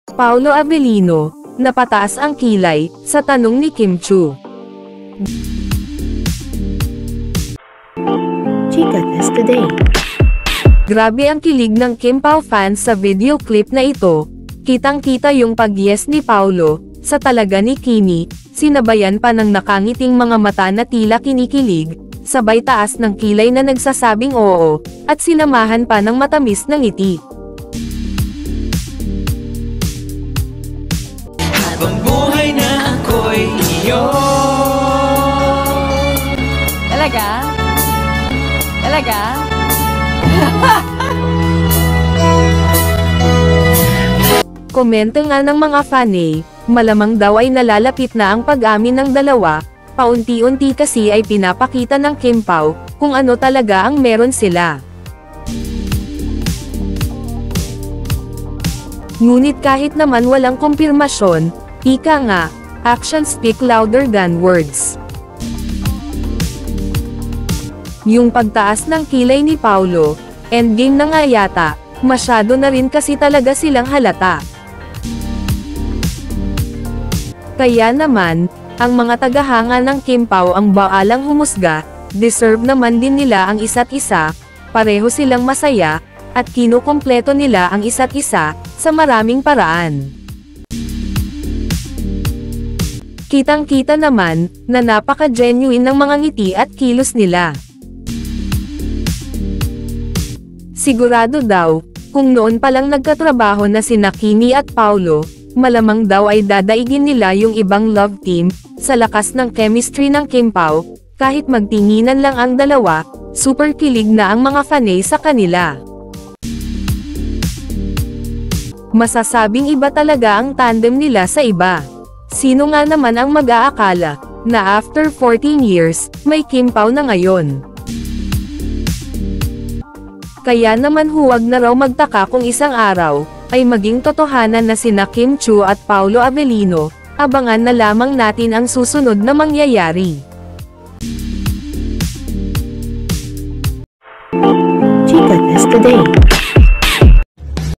HALA! REAKSIYON ni PAULO AVELINO HARAP-HARAPAN TUMAAS ang KILAY sa TANONG ni KIM CHIU
Grabe ang kilig ng kimpau fans sa video clip na ito, kitang-kita yung pag yes ni paulo, sa talaga ni kimmy, sinabayan pa ng nakangiting mga mata na tila kinikilig, sabay taas ng kilay na nagsasabing oo, at sinamahan pa ng matamis na ngiti.

Salamat po sa lahat ng sumusoporta sa channel ko..☺✨